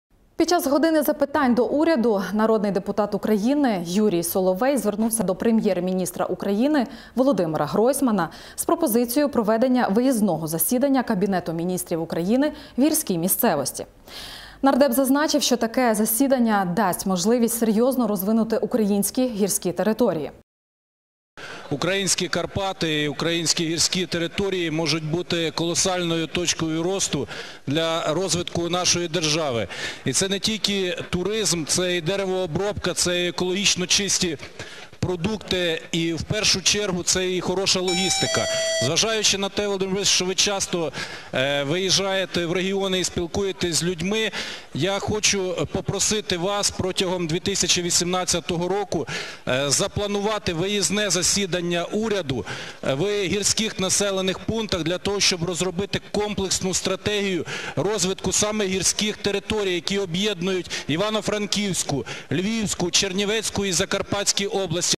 Година запитань до уряду
Під час години запитань до уряду народний депутат України Юрій Соловей звернувся до Прем'єр-міністра України Володимира Гройсмана з пропозицією проведення виїзного засідання Кабінету Міністрів України в гірській місцевості.

Нардеп зазначив, що таке засідання дасть можливість серйозно розвинути українські гірські території.